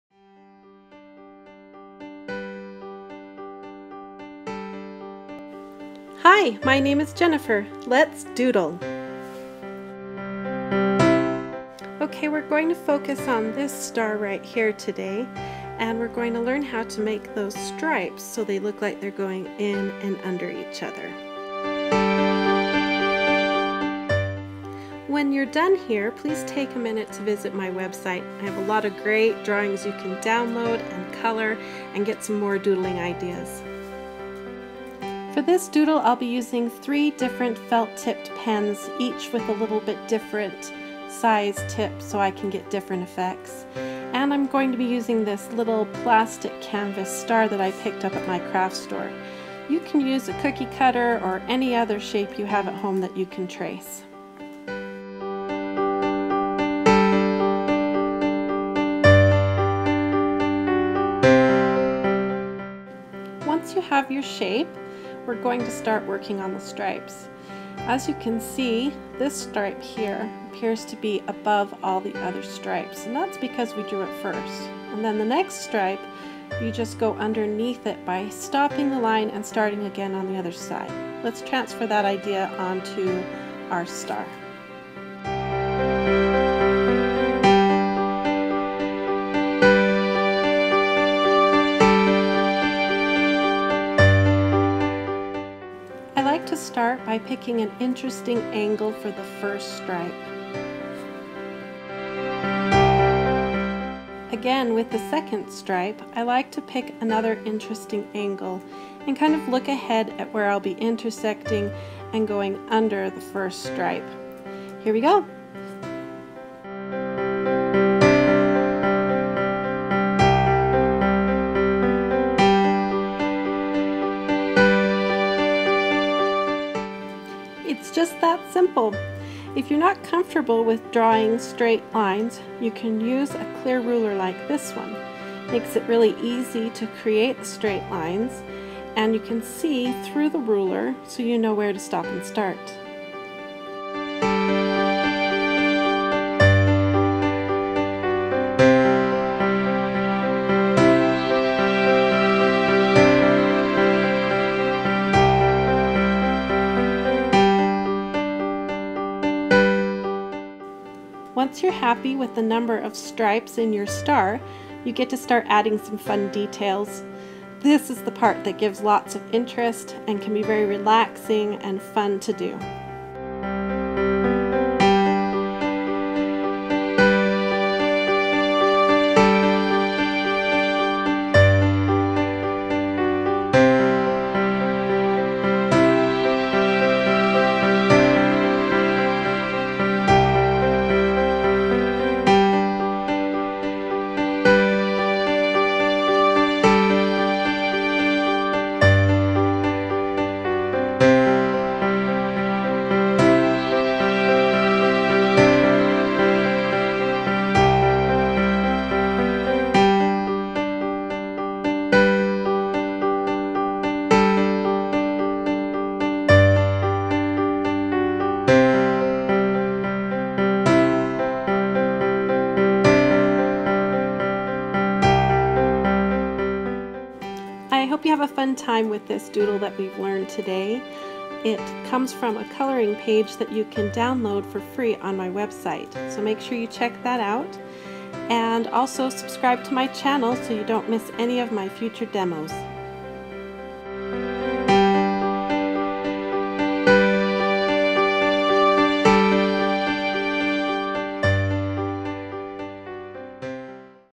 Drawing Tutorial: Stars with Stripes
Jennifer Stay of Coloring Pages Bliss teaches you how to draw overlapping stripes in this doodle demonstration. Family friendly video.

Download the Zentangle inspired coloring page shown in this video